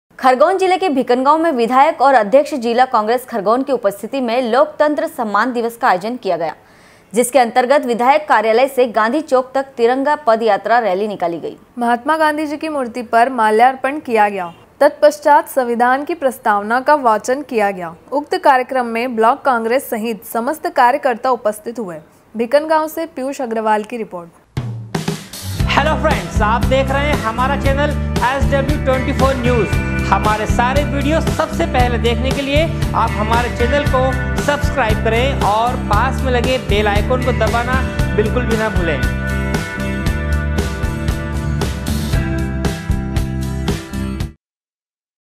bhikangaon- अध्यक्ष जिला कांग्रेस खरगोन की उपस्थिती में लोकतंत्र सम्मान दिवस का आयोजन किया गया
खरगोन जिले के भीकनगांव में विधायक और अध्यक्ष जिला कांग्रेस खरगोन की उपस्थिती में लोकतंत्र सम्मान दिवस का आयोजन किया गया जिसके अन्तर्गत विधायक कार्यालय से गाँधी चैक तक तिरंगा पद यात्रा रैली निकाली गई। भिकनगावॅ से पियूष अग्रवाल की रिर्पोट